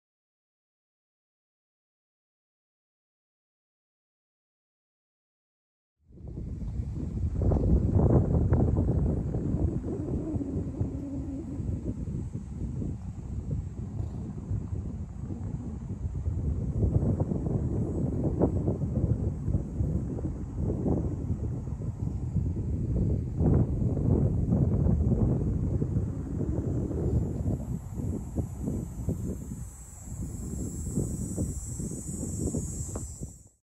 Awesome Pelecinid Wasp visited me on a picnic 8-31-24
This is a clip of an awesome Pelecinid Wasp that visited me while I was having a picnic in Eagan, MN.